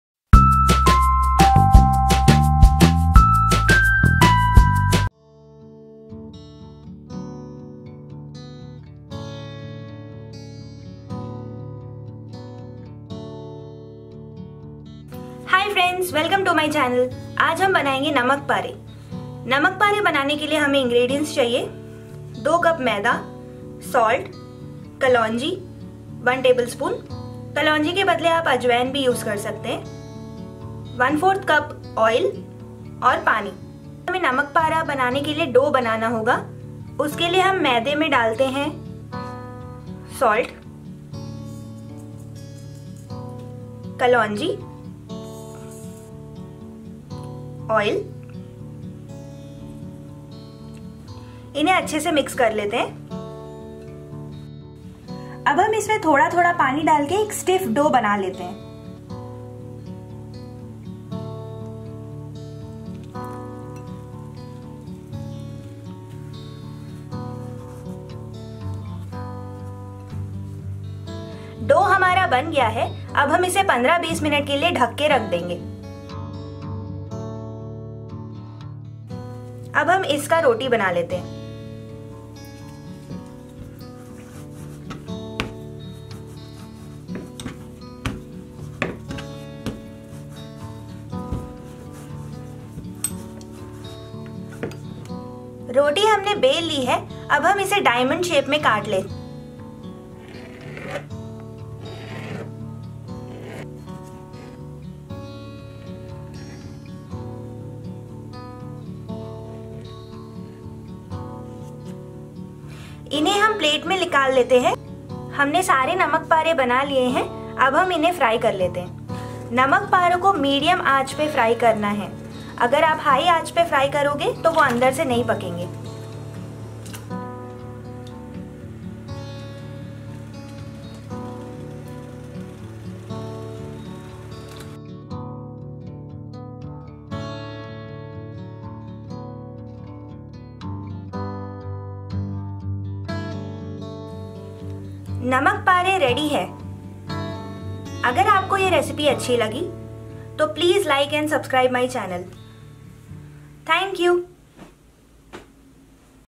Namak Pare
Namak Para is a savoury snack which goes really well with evening tea..It is mainly prepared during Diwali in India..They are also called as nimki, namkin..,etc..The main ingredients are maida(refined flour), ajwain(caraway) seeds or nigella(kalounji) seeds and are deep fried in oil..